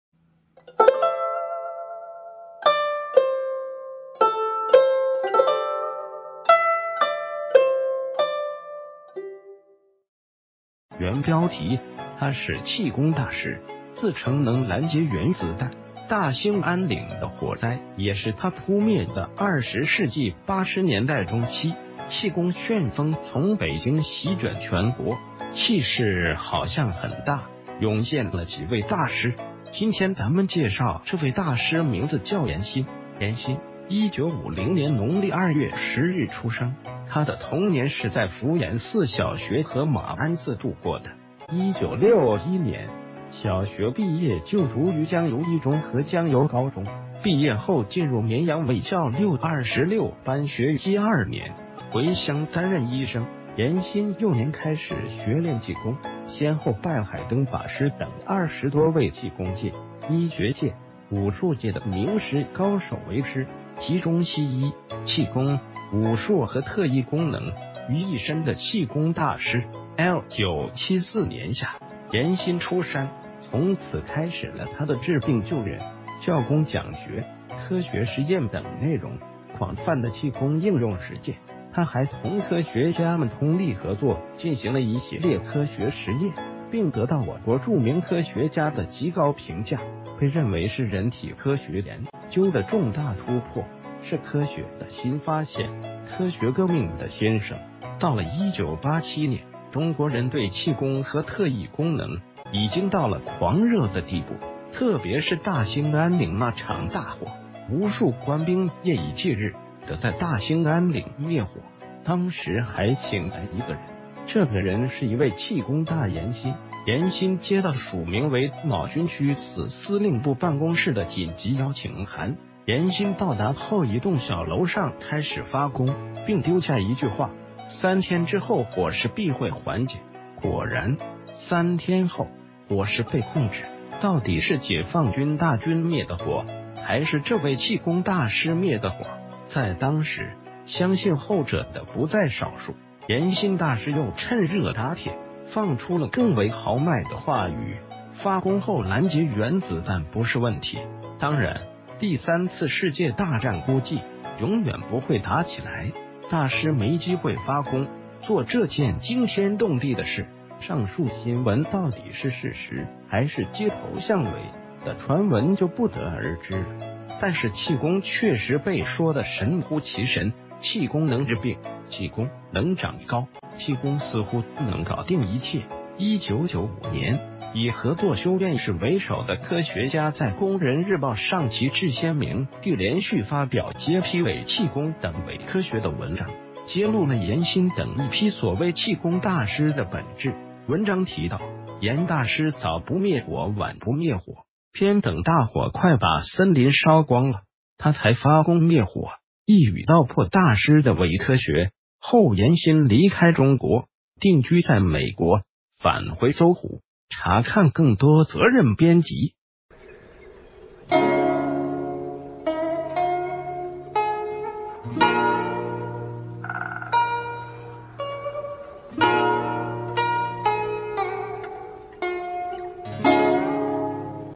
他是“氣功大師”自稱能攔截原子彈，大興安嶺的火災也是他撲滅的_搜狐歷史_搜狐網
原標題：他是“氣功大...